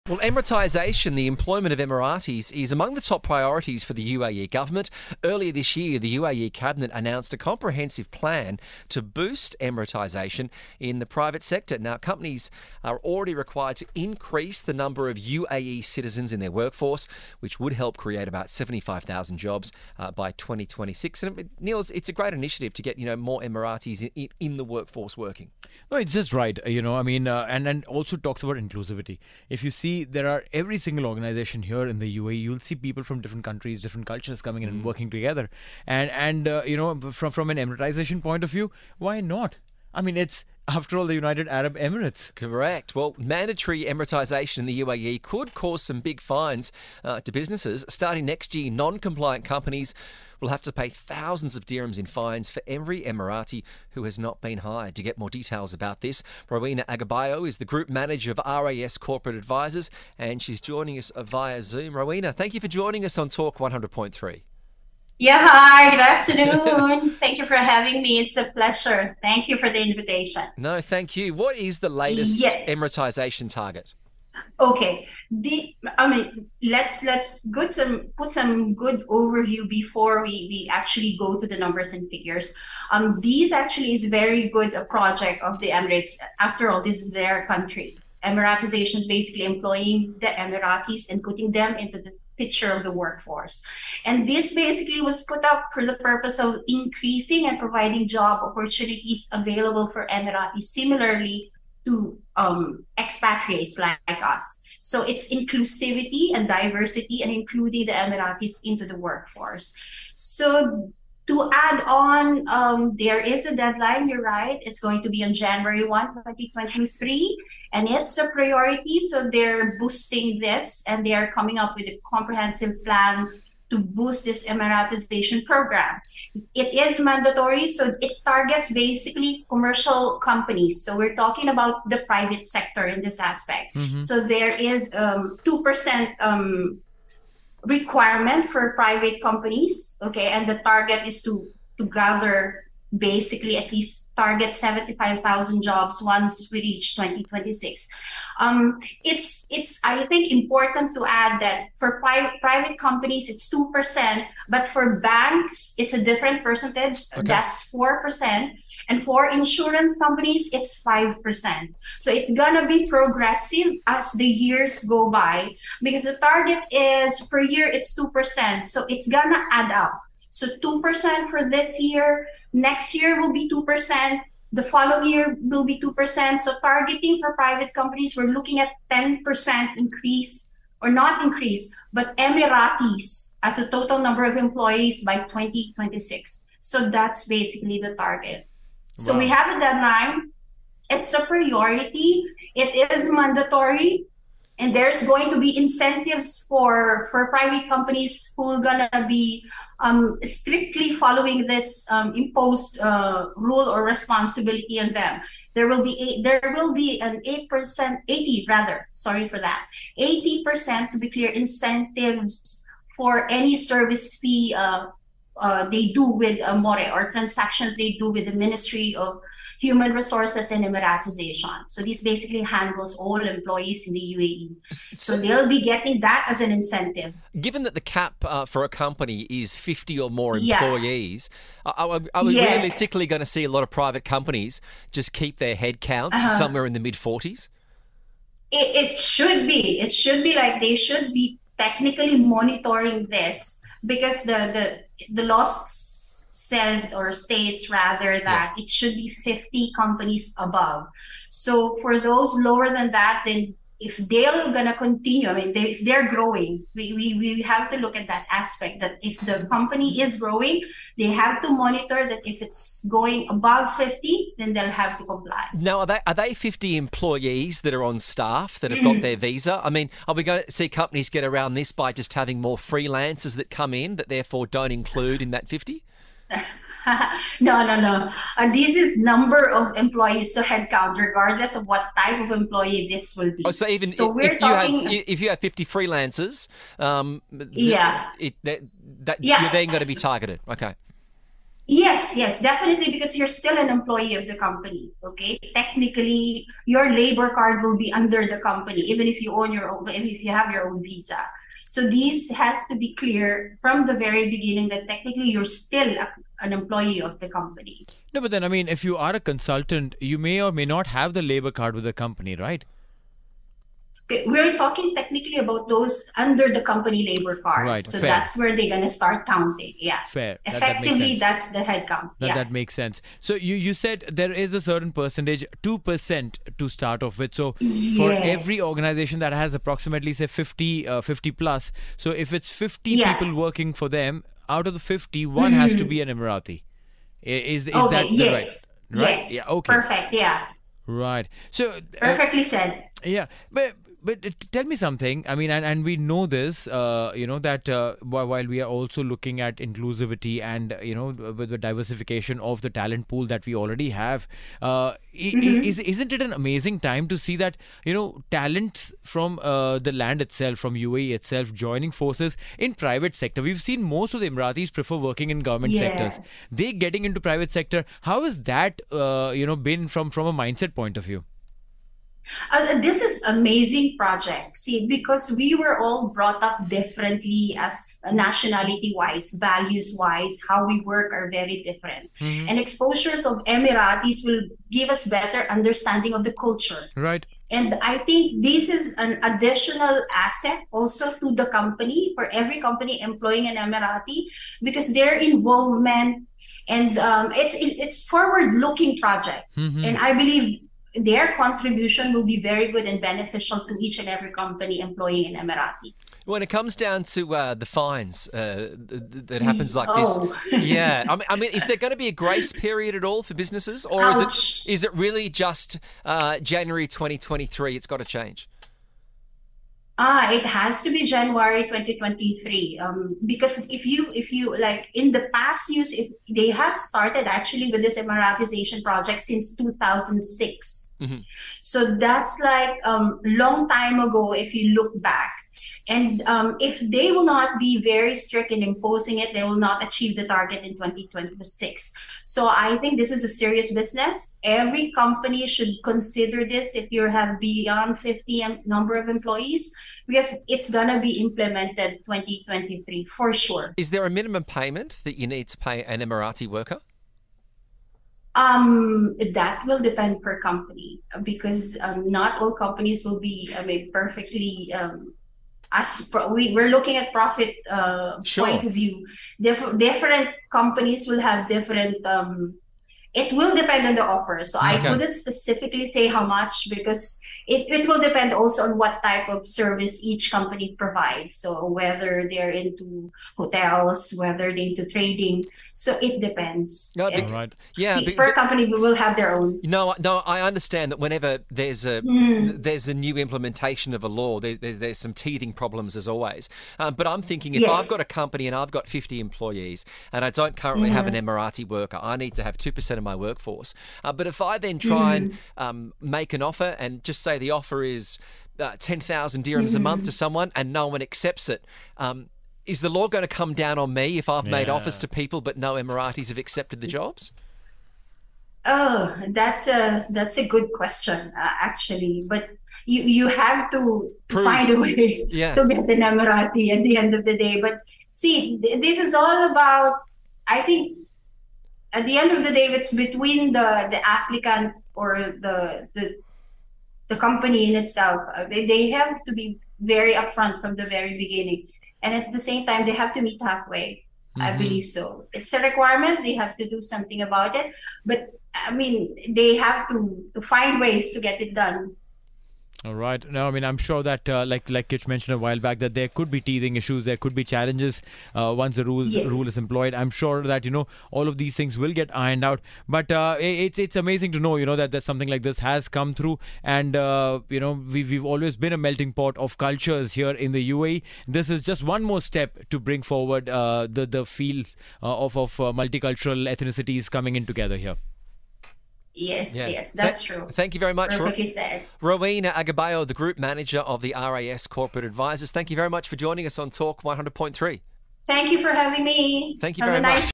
2 per cent of a company's skilled staff must be Emirati. Rowena Aggabao from RAS Corporate Adviso...
Companies in the UAE with 50 or more employees are mandated to annually raise their Emiratization rate by 2 per cent of overall skilled jobs. By the end of this year, 2 per cent of a company’s skilled staff must be Emiratis. This must increase by 2 per cent every year, to reach 10 per cent by 2026.&nbsp;

Rowena Aggabao is the Group Manager of RAS Corporate Advisors and spoke with TSB. &nbsp;